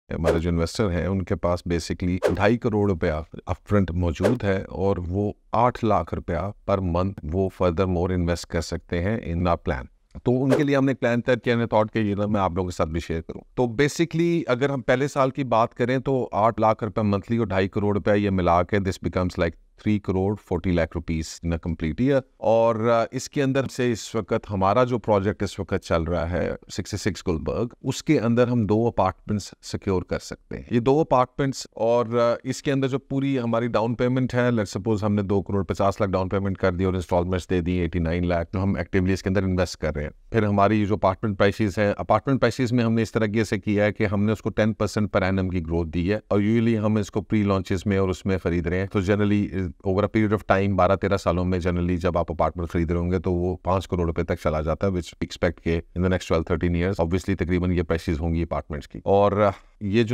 Strategic Investment Plan for Sixty6 Gulberg Apartments (Part 1) - REAL ESTATE LAHORE
📍 Address: 147 CCA-Phase 5, DHA Lahore.